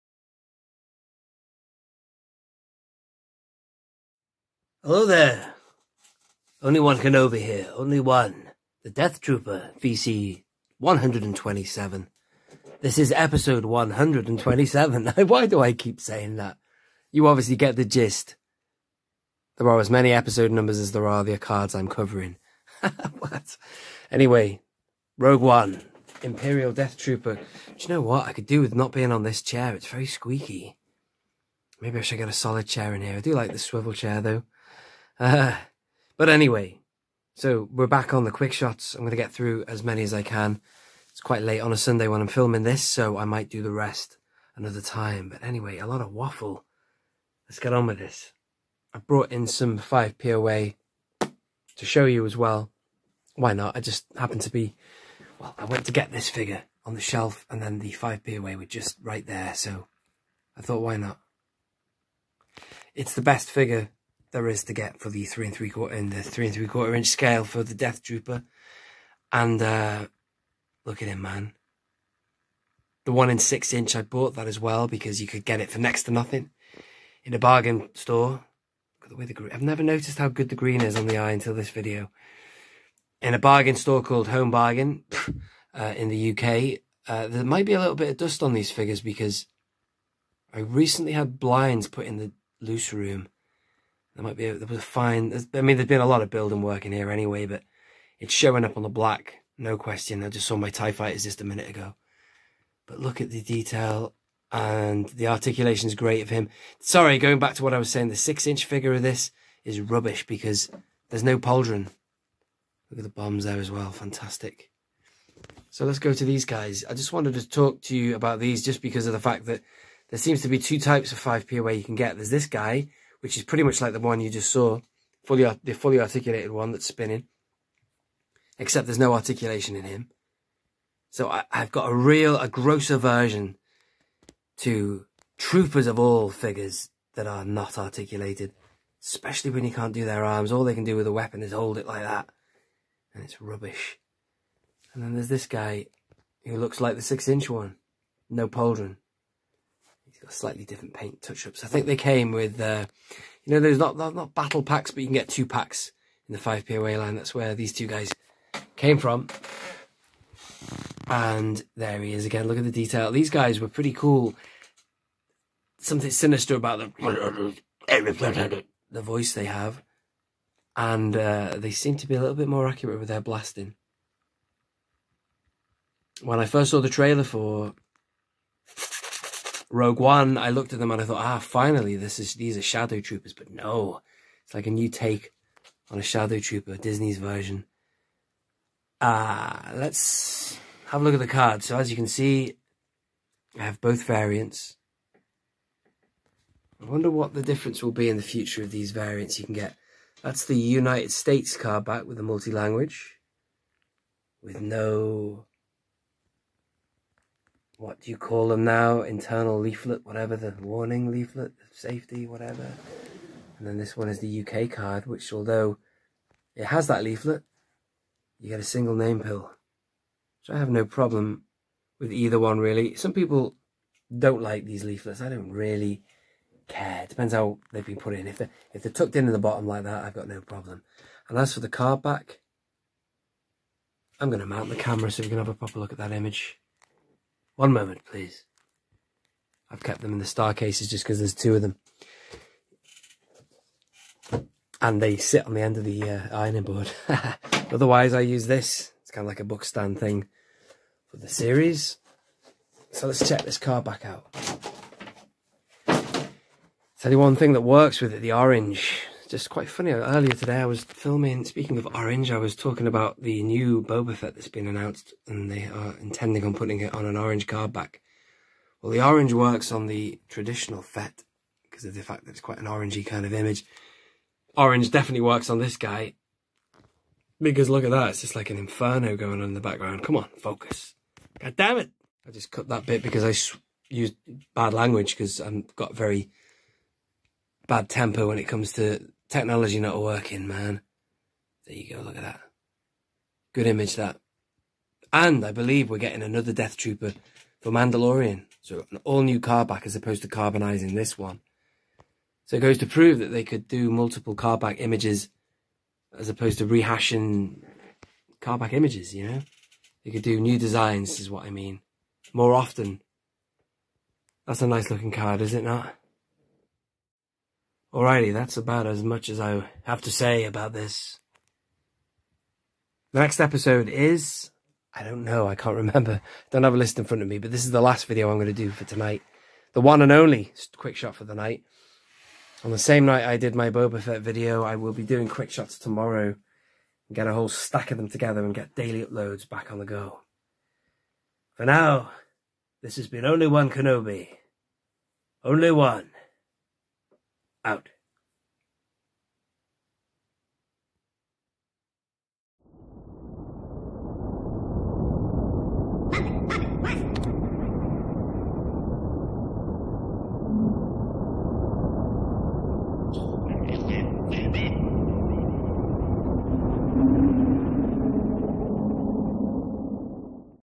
VC127 Imperial Death Trooper | Star Wars Vintage Collection 3.75 QUICKSHOTS
As seen in Rogue One and even The Mandalorian, the sinister Imperial Death Trooper - VC127 of the Star Wars Vintage Collection! O.O.K 👌🏼💙